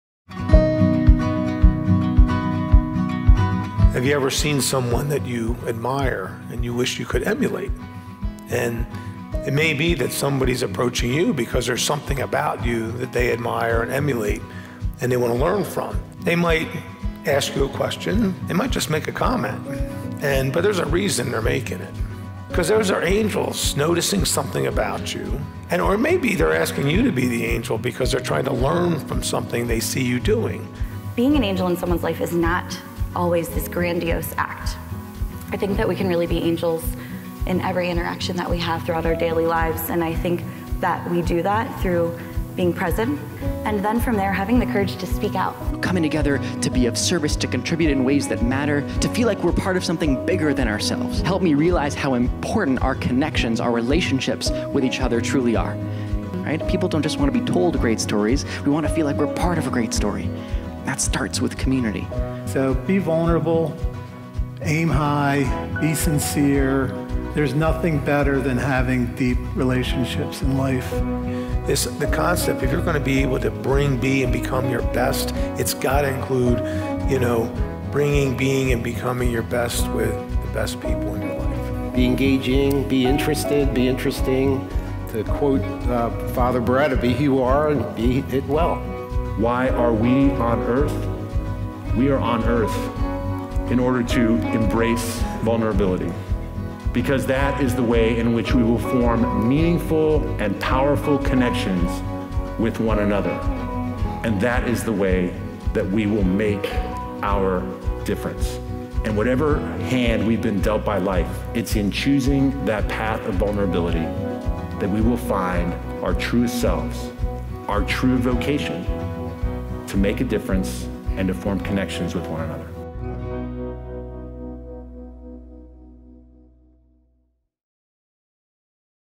The Power of Being Vulnerable | Building Compelling Connections
Siegfried's MY Journey® program helps people develop themselves personally and professionally in a variety of areas. This event was focused on developing mastery in Compelling Connections, which helps you build meaningful relationships by being vulnerable, having a clear understanding of self, and your impact on those around you.

Featuring Guest Speakers - Shane Feldman, Entrepreneur, Jessie Funk, Musical Artist & Motivational Speaker, & Fr. Chris Beretta, OSFS, Principal of Salesianum School